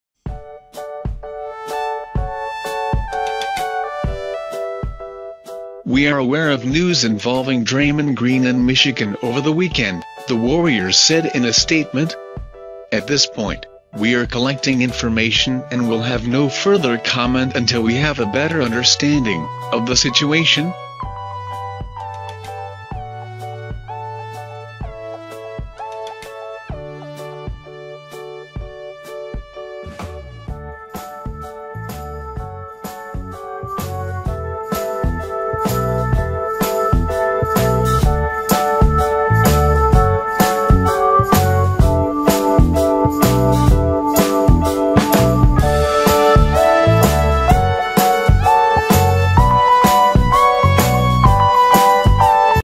Golden State Warriors - Draymond Green arrested on suspicion of assault - HD
Golden State Warriors forward Draymond Green was arrested on suspicion of assault Sunday in Michigan, according to court records.

The records reviewed by USA TODAY Sports show East Lansing police arrested Green on a misdemeanor assault and battery charge at 2:28 am Sunday. Green was released on a $200 bond and he's scheduled to be arraigned on July 20 .

“We are aware of news involving Draymond Green in Michigan over the weekend,” the Warriors said in a statement. “At this point, we are collecting information and will have no further comment until we have a better understanding of the situation.”

Green faces a maximum of 93 days in jail and a fine of up to $500.  MLive.com was the first to report assault charge on Monday, citing sources with knowledge of the arrest.

"The police department is not confirming any arrest with that name," East Lansing police spokesperson Lt. Scott Wrigglesworth told MLive.com. "What I can confirm is that we did arrest a current NBA basketball player this weekend and he was arrested for assault. That's all we're confirming right now."

Messages left with Wrigglesworth by USA TODAY Sports were not immediately returned.

Green is a member of the U.S. Men’s Olympic men’s basketball team and is scheduled to attend Team USA’s camp in Las Vegas July 18-21.